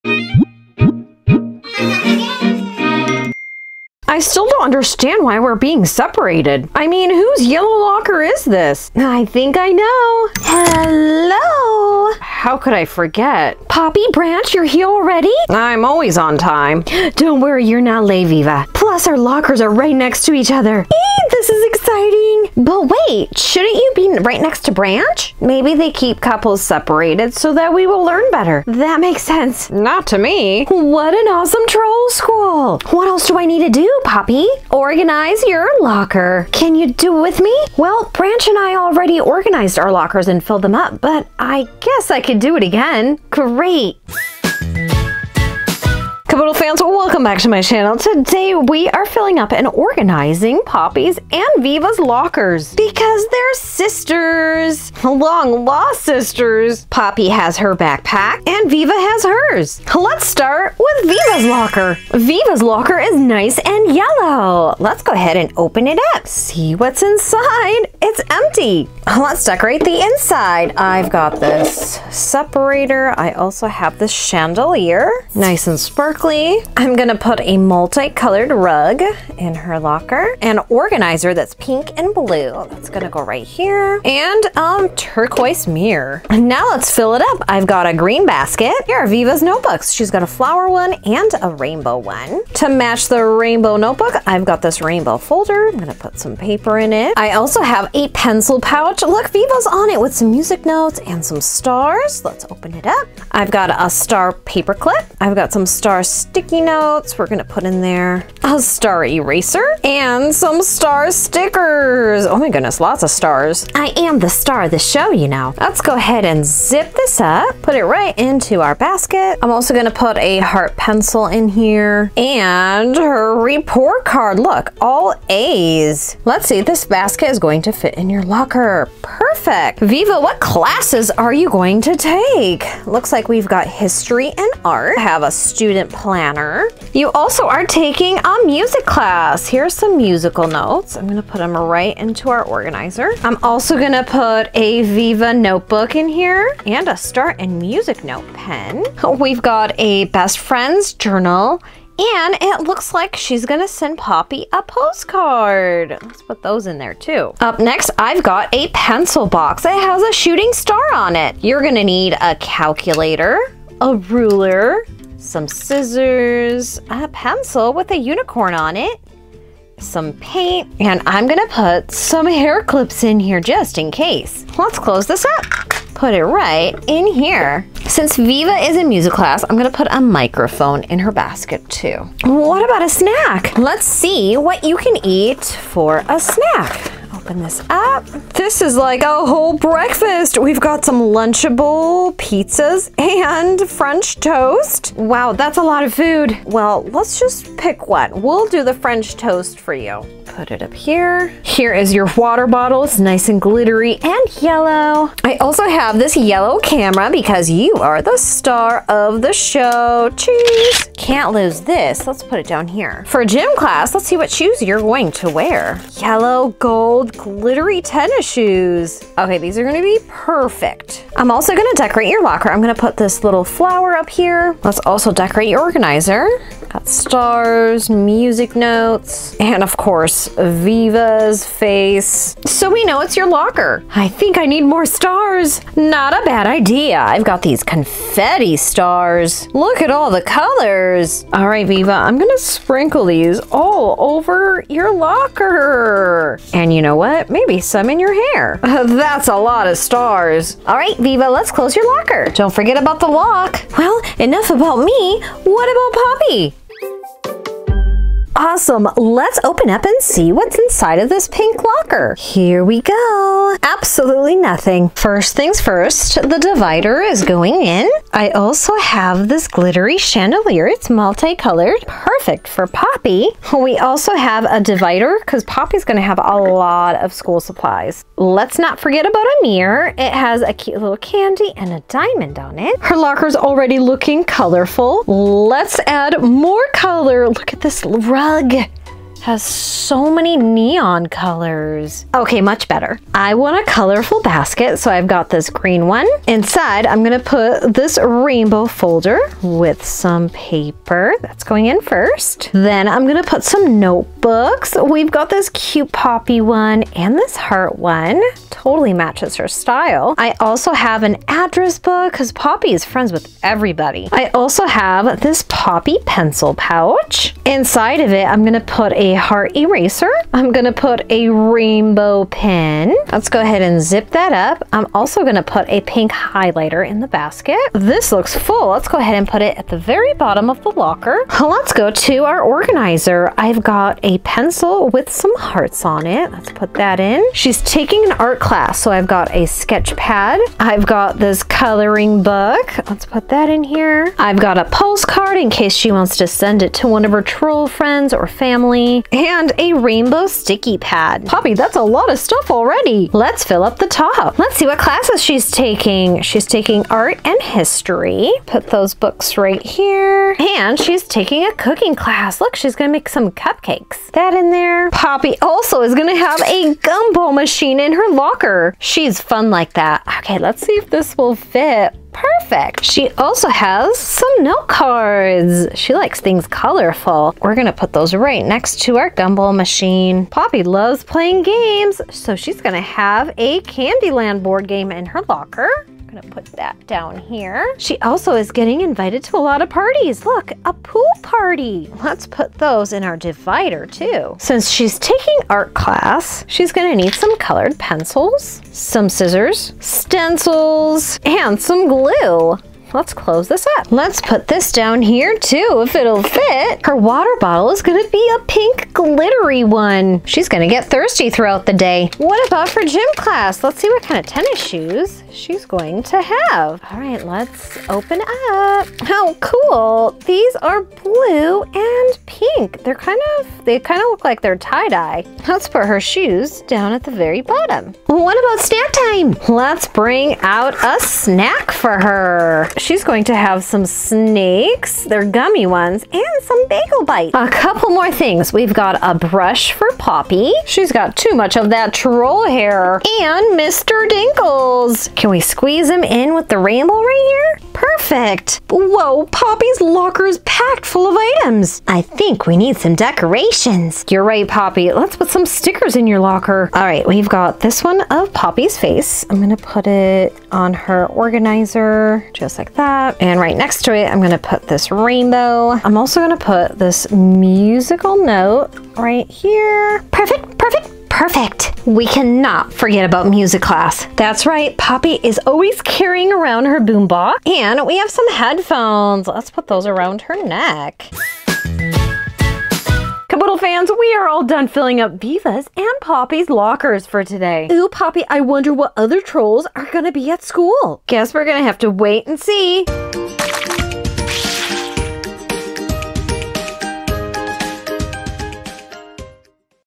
The Trolls Band Together Movie DIY Custom Back to School Locker Organization with Poppy and Viva
Lets organize with The Trolls Band Together movie DIY custom back to school lockers including Poppy and Viva dolls during their school day. We also organize and decorate the lockers with lots of fun stationary items, stickers, and items that they need. Which locker did you like. What a fun pretend play.